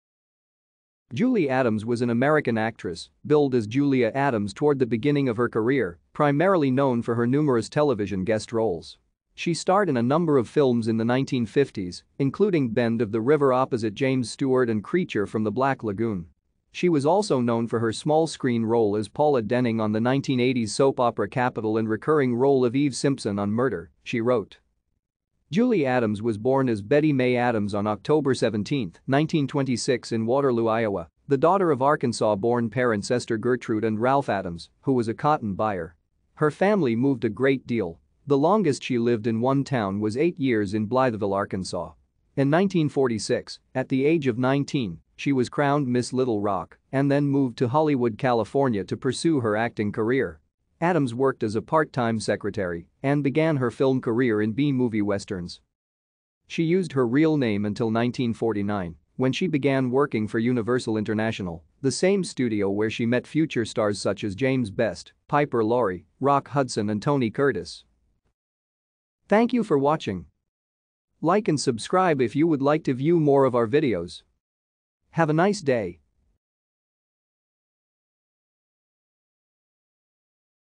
Julie Adams
Julie Adams  was an American actress, billed as Julia Adams toward the beginning of her career, primarily known for her numerous  guest roles. She starred in a number of s in the 1950s, including Bend of the River  opposite James Stewart and Creature from the Black Lagoon. She was also known for her small screen role as Paula Denning on the 1980s soap opera Capitol and recurring role of Eve Simpson on Murder, She Wrote.